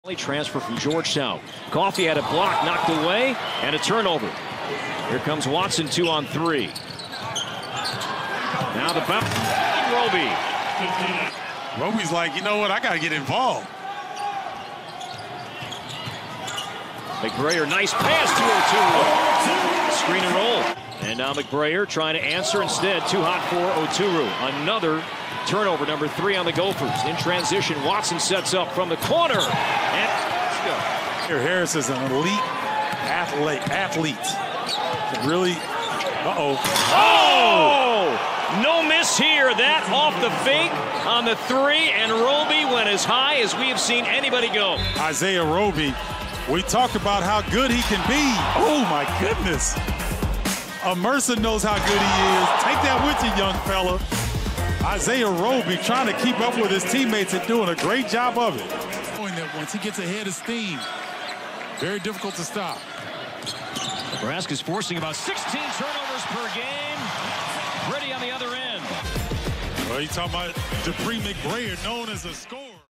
First Half Highlights: Nebraska at Minnesota | Big Ten Basketball
The top plays of the first half.